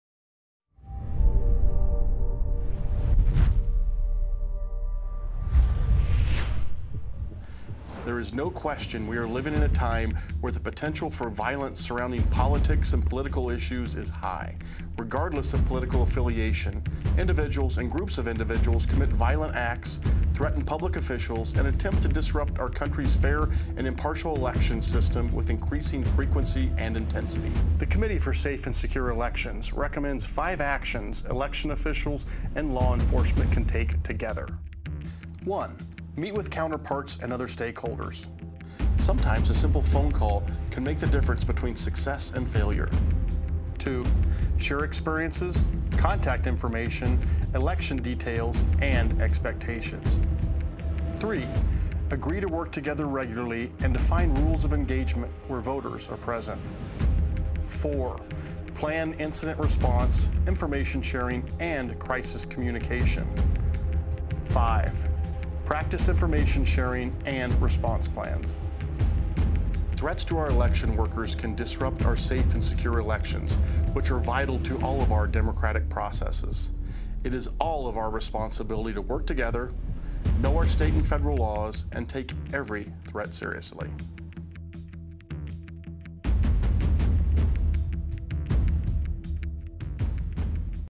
Five Steps to Protect Our Elections from the Committee for Safe and Secure Elections
The Committee for Safe and Secure Elections (CSSE) released this 5-step guide for law enforcement and election officials ahead of the 2022 midterms to help protect voters and elections officials from violence.

CSSE features cross-partisan experts in election administration and law enforcement who aim to support policies and practices that protect election workers and voters from violence, threats, and intimidation. The Committee also works to build relationships and trust between election officials and law enforcement to better equip both to prevent and respond to threats and violence against voters and election workers.

(The Committee for Safe and Secure Elections is supported by the Brennan Center for Justice at NYU Law, The Elections Group, Protect Democracy, and the R Street Institute.)